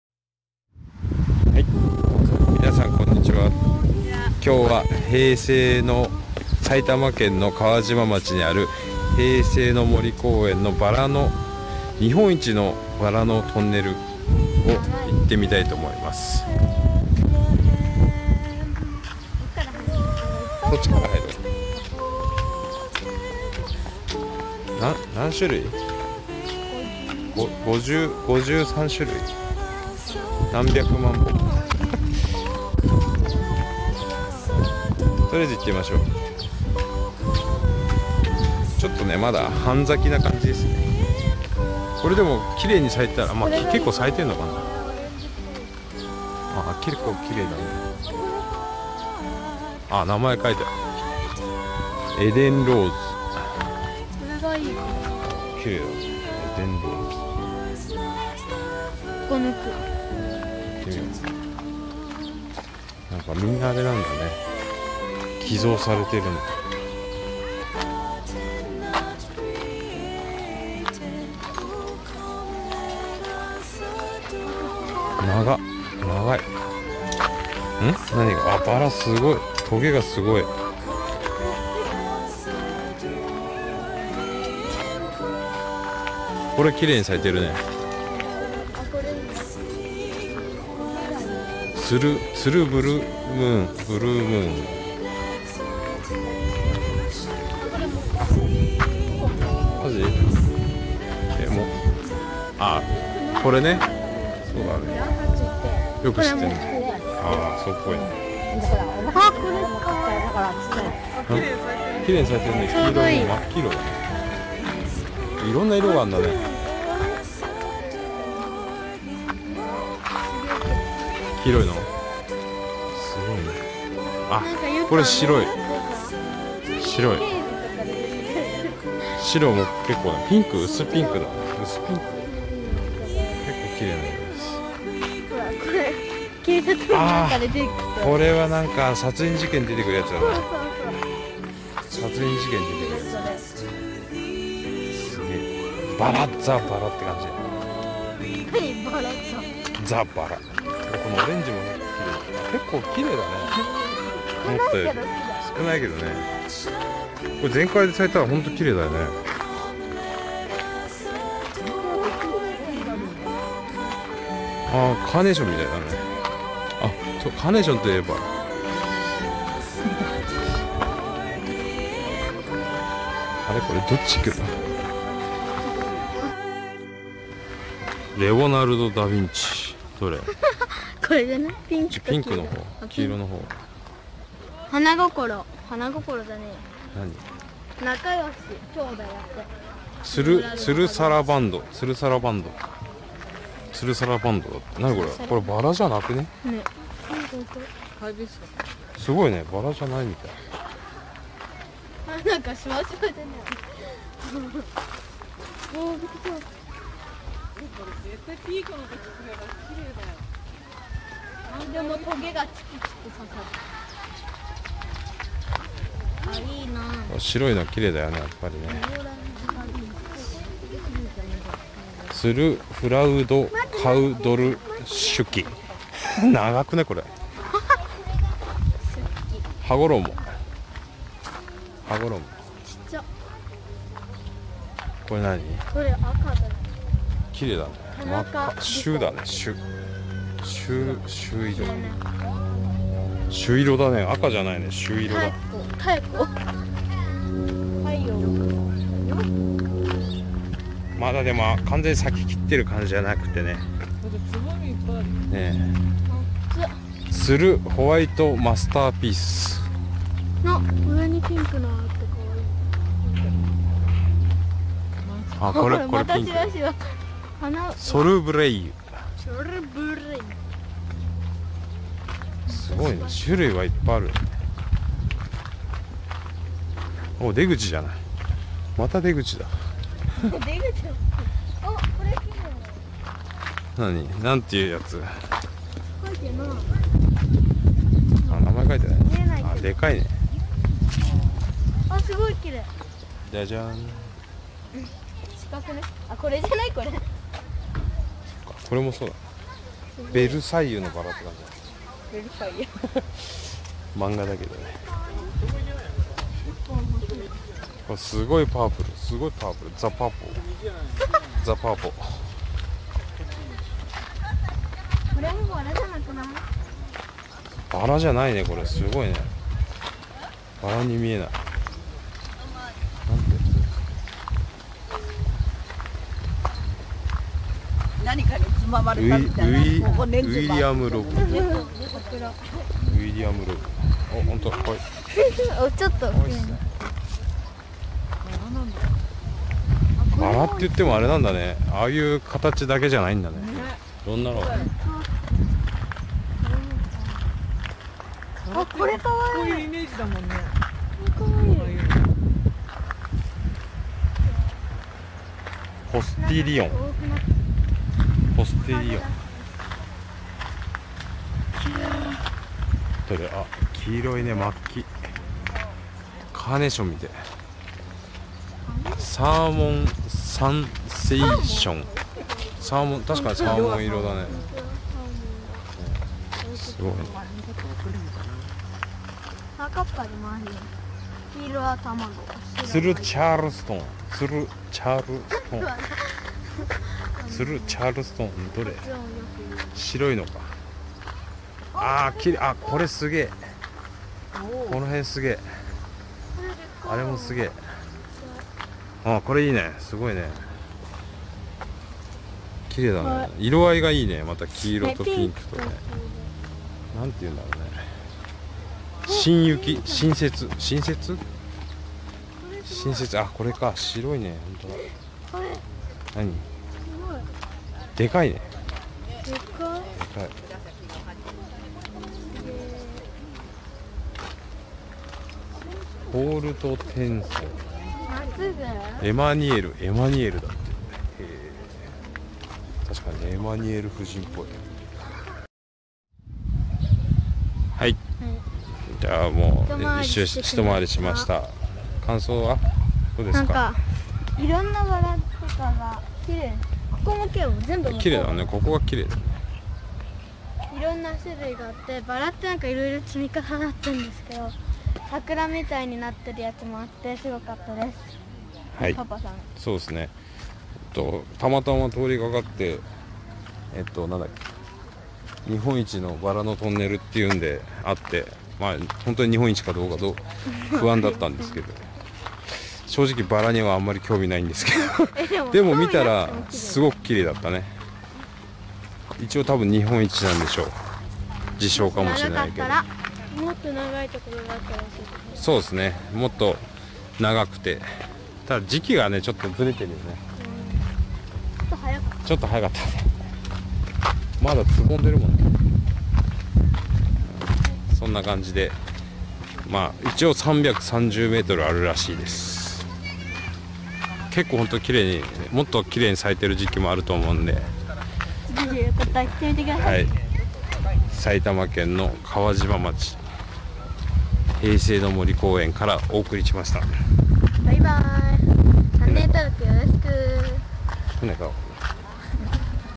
【日本一】バラのトンネル。日本一長い。 Japanese rose tunnel
日本一長いバラのトンネル 埼玉県川島町、平成の森公園、Japanese rose tunnel
凄い動画…
はじめしゃちょーさん